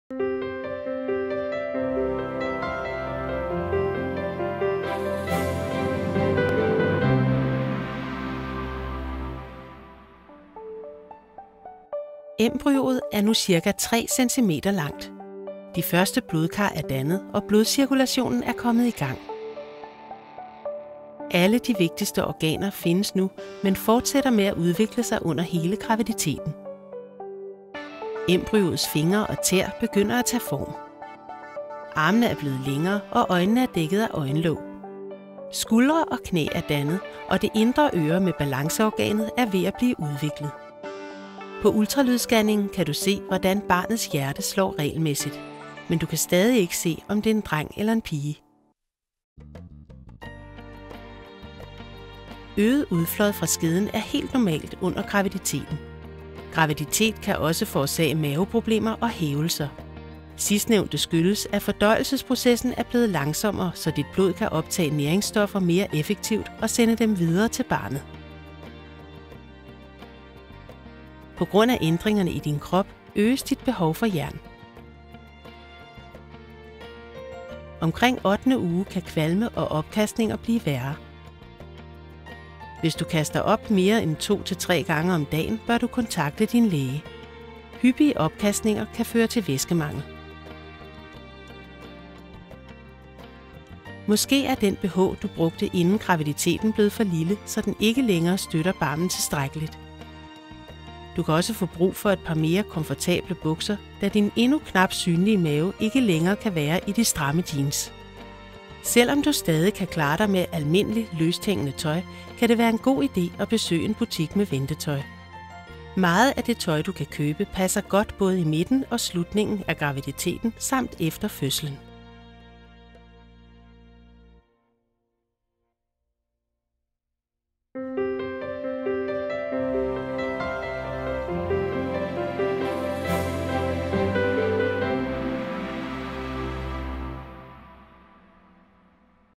Graviditet uge 8
Embryoet er nu cirka 3 centimeter langt. De første blodkar er dannet, og blodcirkulationen er kommet i gang. Alle de vigtigste organer findes nu men fortsætter med at udvikle sig under hele graviditeten.
Embryoets fingre og tæer begynder at tage form. Armene er blevet længere, og øjnene er dækket af øjenlåg. Skuldre og knæ er dannet, og det indre øre med balanceorganet er ved at blive udviklet. På ultralydsscanningen kan du se, hvordan barnets hjerte slår regelmæssigt. Men du kan stadig ikke se, om det er en dreng eller en pige.

Øget udflåd fra skeden er helt normalt under graviditeten. Graviditet kan også forårsage maveproblemer og hævelser. Sidstnævnte skyldes, at fordøjelsesprocessen er blevet langsommere, så dit blod kan optage næringsstoffer mere effektivt og sende dem videre til barnet.

På grund af ændringerne i din krop, øges dit behov for jern. Omkring ottende uge kan kvalme og opkastninger blive værre. Hvis du kaster op mere end 2 - 3 gange om dagen, bør du kontakte din læge. Hyppige opkastninger kan føre til væskemangel.

Måske er den BH, du brugte inden graviditeten, blevet for lille, så den ikke længere støtter barmen tilstrækkeligt. Du kan også få brug for et par mere komfortable bukser, da din endnu knap synlige mave ikke længere kan være i de stramme jeans.

Selvom du stadig kan klare dig med almindeligt, løsthængende tøj, kan det være en god ide at besøge en butik med ventetøj.
Meget af det tøj, du kan købe, passer godt både i midten og slutningen af graviditeten samt efter fødslen.